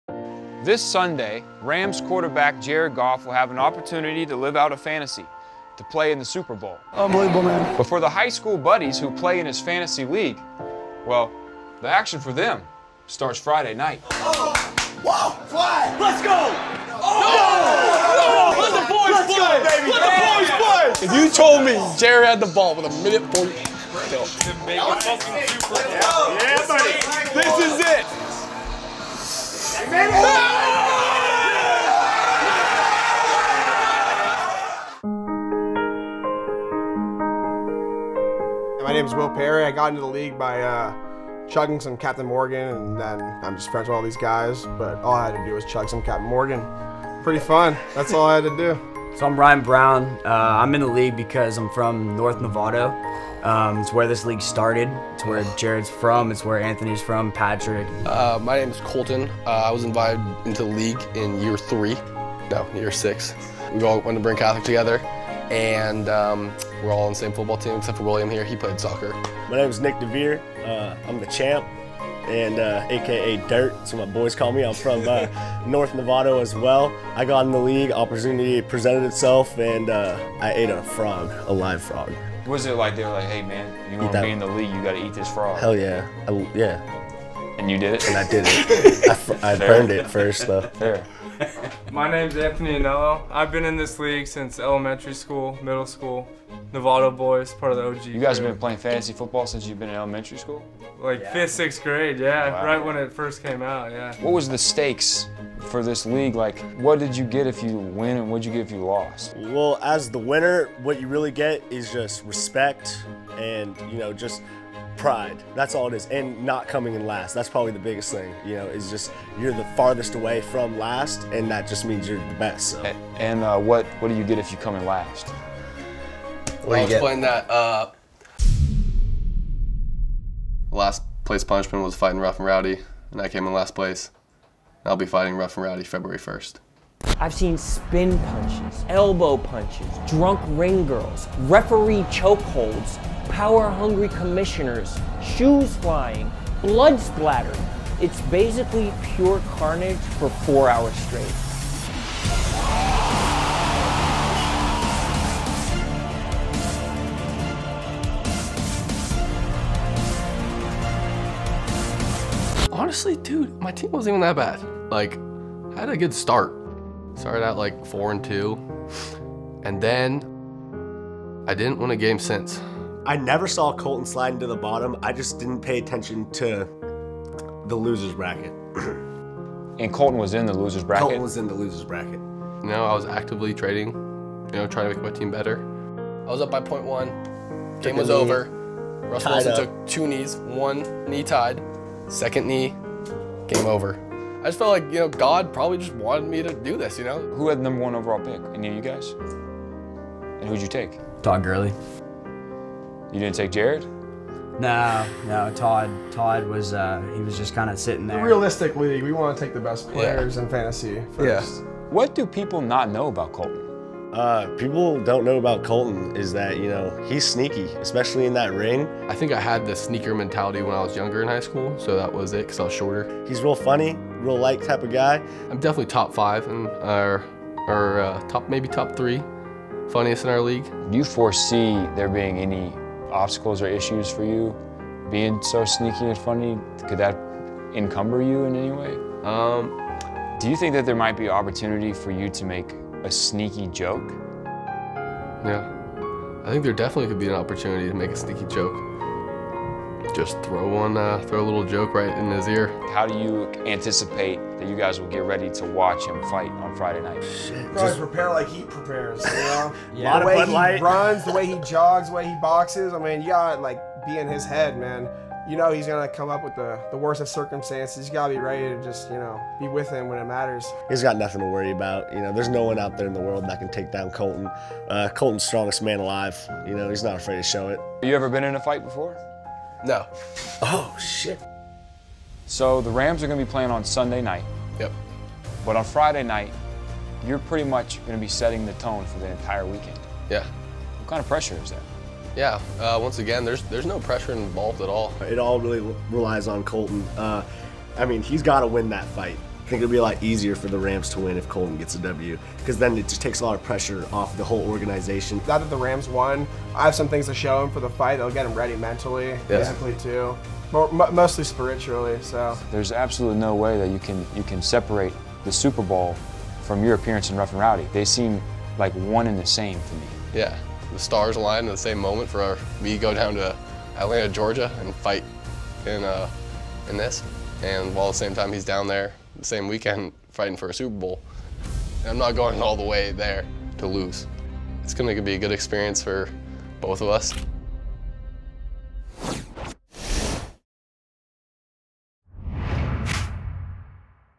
Jared Goff's Fantasy League Loser Has To Fight in a Boxing Match This Friday
Don’t miss RNR7 this Friday in Atlanta at the Super Bowl

Jared Goff & co. live life on the edge. Their high stakes fantasy league has landed one man in the Super Bowl Rough N Rowdy ring. Will he set the tone for the weekend? Or will Jared have to go into Sunday with a bad taste in his mouth? Watch Rough N Rowdy Friday night live from Atlanta to find out.